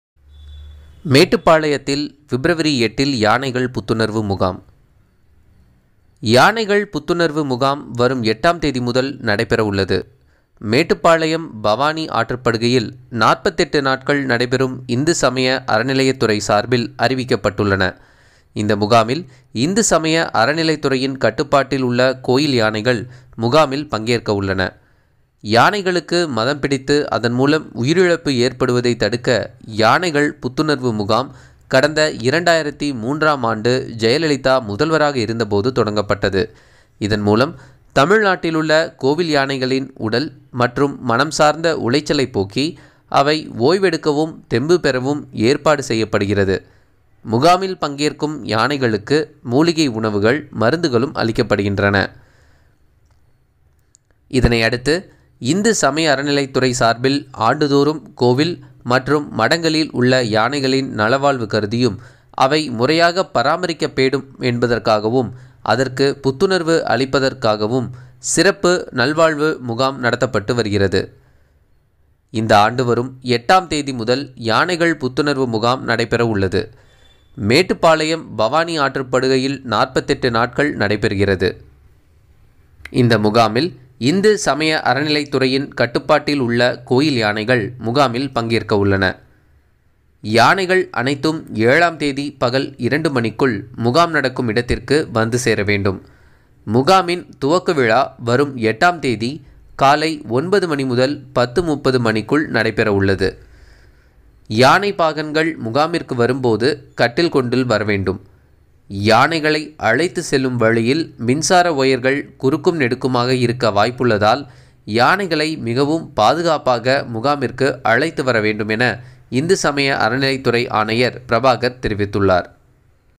மேட்டுப்பாளையத்தில் பிப்.8ல் யானைகள் புத்துணர்வு முகாம்|Elephants Camp @ Mettuppalayam Hello City Tv
மேட்டுப்பாளையத்தில் பிப்.8ல் யானைகள் புத்துணர்வு முகாம் : தமிழக அரசு அறிவிப்பு

கோவை: யானைகள் புத்துணர்வு முகாம் வரும் 8ஆம் தேதி முதல் நடைபெற உள்ளது. மேட்டுப்பாளையம் பவானி ஆற்றுப்படுகையில் 48 நாட்கள் நடைபெறும் இந்து சமய அறநிலையத்துறை சார்பில் அறிவிக்கப்பட்டுள்ளன. இந்த முகாமில் இந்து சமய அறநிலையத்துறையின் கட்டுப்பாட்டில் உள்ள கோயில் யானைகள் முகாமில் பங்கேற்க உள்ளன. யானைகளுக்கு மதம் பிடித்து அதன் மூலம் உயிரிழப்பு ஏற்படுவதை தடுக்க யானைகள் புத்துணர்வு முகாம் கடந்த 2003ஆம் ஆண்டு ஜெயலலிதா முதல்வராக இருந்த போது தொடங்கப்பட்டது. இதன் மூலம் தமிழ்நாட்டிலுள்ள கோவில் யானைகளின் உடல் மற்றும் மனம் சார்ந்த உளைச்சலைப் போக்கி, அவை ஓய்வெடுக்கவும் தெம்பு பெறவும் ஏற்பாடு செய்யப்படுகிறது. முகாமில் பங்கேற்கும் யானைகளுக்கு மூலிகை உணவுகள், மருந்துகளும் அளிக்கப்படுகின்றன. இதனையடுத்து இந்து சமய அறநிலையத்துறை சார்பில் ஆண்டுதோறும் கோவில் மற்றும் மடங்களில் உள்ள யானைகளின் நலவாழ்வு கருதியும், அவை முறையாக பராமரிக்கப்பட வேண்டும் என்பதற்காகவும் அதற்கு புத்துணர்வு அளிப்பதற்காகவும் சிறப்பு நலவாழ்வு முகாம் நடத்தப்பட்டு வருகிறது. இந்த ஆண்டு வரும் 8ஆம் தேதி முதல் யானைகள் புத்துணர்வு முகாம் நடைபெற உள்ளது. மேட்டுப்பாளையம் பவானி ஆற்றுப்படுகையில் 48 நாட்கள் நடைபெறுகிறது. இந்த முகாமில் இந்து சமய அறநிலையத்துறையின் கட்டுப்பாட்டில் உள்ள கோயில் யானைகள் முகாமில் பங்கேற்க உள்ளன. யானைகள் அனைத்தும் 7ஆம் தேதி பகல் 2 மணிக்குள் முகாம் நடக்கும் இடத்திற்கு வந்து சேர வேண்டும். முகாமின் துவக்க விழா வரும் 8ஆம் தேதி காலை 9 மணி முதல் 10.30 மணிக்குள் நடைபெற உள்ளது. யானை பாகன்கள் முகாமிற்கு வரும் போது கட்டில் கொண்டு வர வேண்டும். யானைகளை அழைத்து செல்லும் வழியில் மின்சார ஒயர்கள் குறுக்கும் நெடுக்குமாக இருக்க வாய்ப்புள்ளதால் யானைகளை மிகவும் பாதுகாப்பாக முகாமிற்கு அழைத்து வர வேண்டும் என இந்து சமய அறநிலையத்துறை ஆணையர் பிரபாகர் தெரிவித்துள்ளார்.